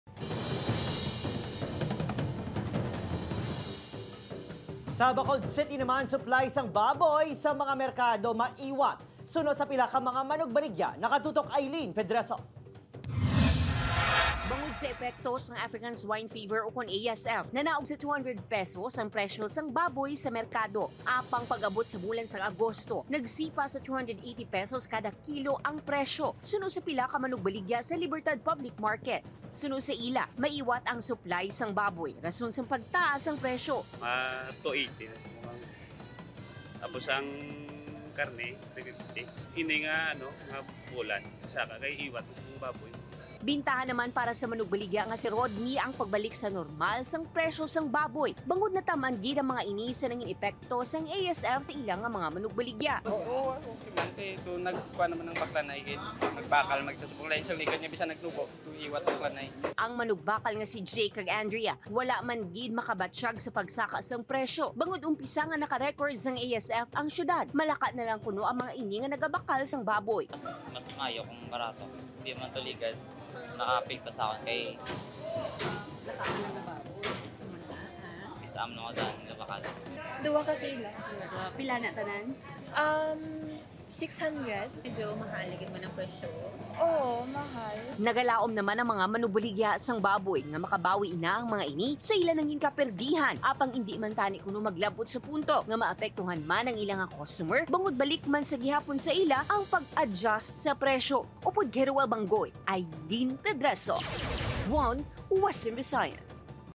One Western Visayas: Presyo sang Karne sang Baboy, Nagsaka na sa P280 Kada Kilo Subong nga Bulan
Suplay sang baboy sa merkado sa Bacolod City maiwat suno sa mga manugbaligya.

Presyo sang karne sang baboy, nagsaka na sa P280 kada kilo subong nga bulan.